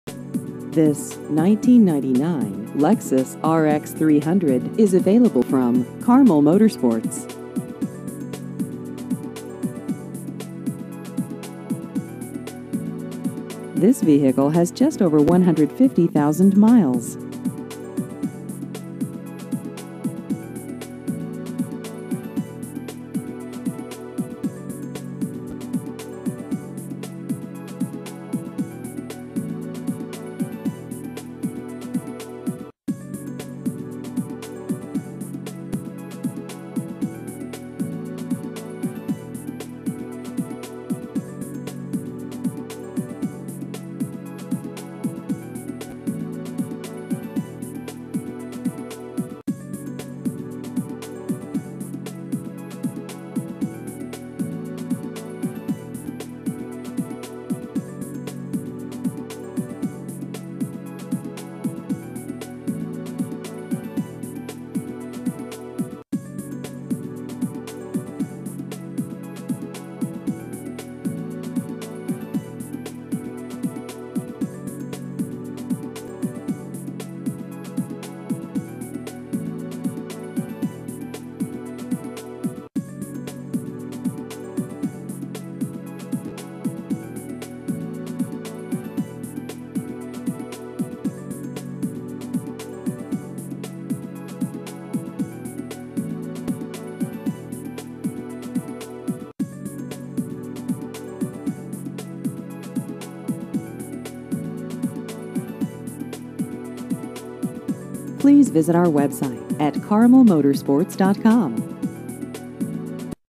1999 Lexus RX 300 Used Cars Indianapolis IN
This 1999 Lexus RX 300 is available from Carmel Motorsports.